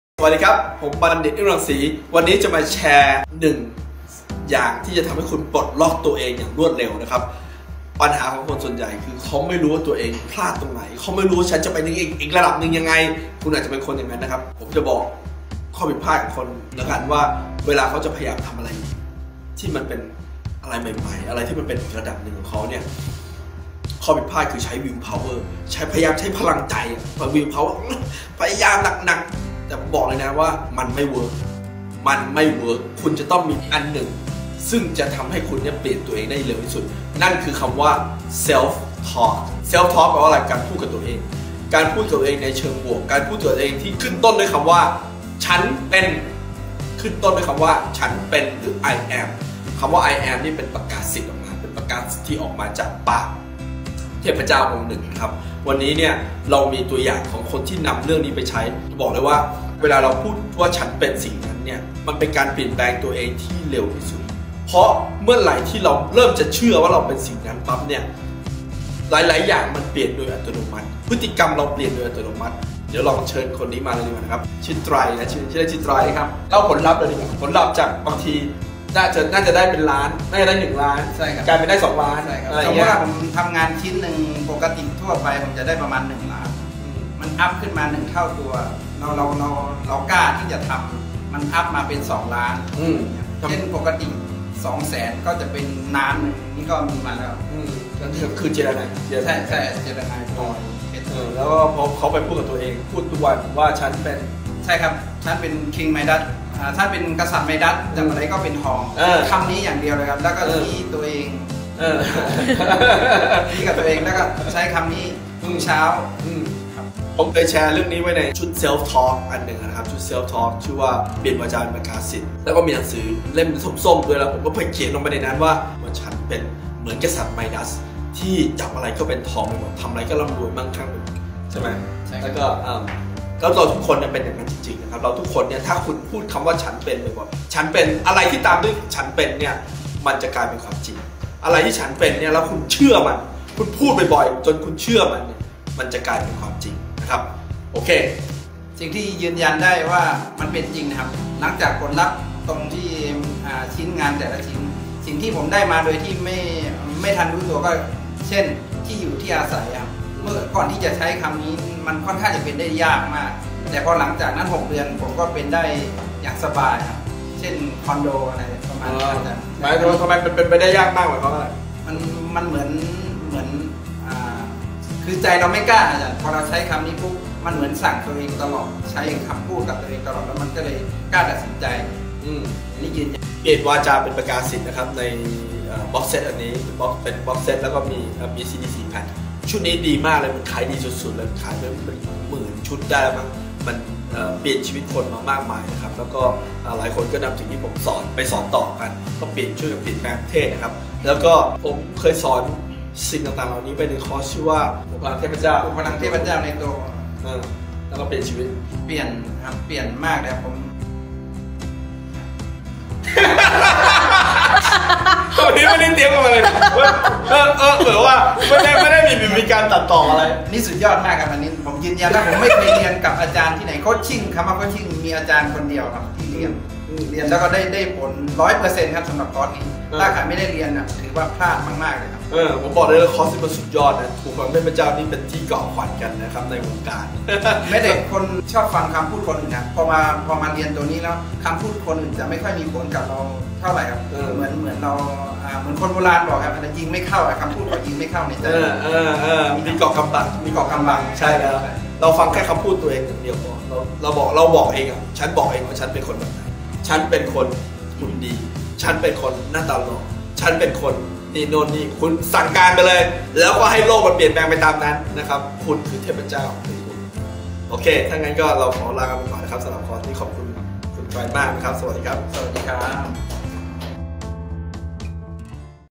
"1 อย่างที่จะทำให้คุณปลดล็อกตัวเอง อย่างรวดเร็ว" | Bundit Ungrangsee
"1 อย่างที่จะทำให้คุณปลดล็อกตัวเอง อย่างรวดเร็ว"
เหตุผลใหญ่ ที่คนส่วนมาก ไม่ก้าวหน้า .. คือ ความเชื่อเก่าๆ
ความเชื่อที่จำกัดตัวเขา
มี 1 อย่าง ที่จะช่วยคุณได้อย่างรวดเร็ว
คลิปนี้ มีแขกรับเชิญเป็นตัวอย่าง ว่าเขาทำอย่างไร
คนนี้ มีสมญานามว่า "กษัตริย์ไมดัส"
เพราะจับอะไรก็เป็น "ทอง" ไปหมด
ก่อนมาเจอผม ทำงานได้ประมาณ ชิ้นละ 1,000,000 บาท
หลังจากเจอผม ได้มากกว่านั้น
คลิปนี้ สรุปหลักคิดให้คุณ
ไปดูกันเลยครับ
ติดตามผลงานผม เพิ่มเติมได้ที่

Line@Bundit หรือกดได้ที่ลิงค์นี้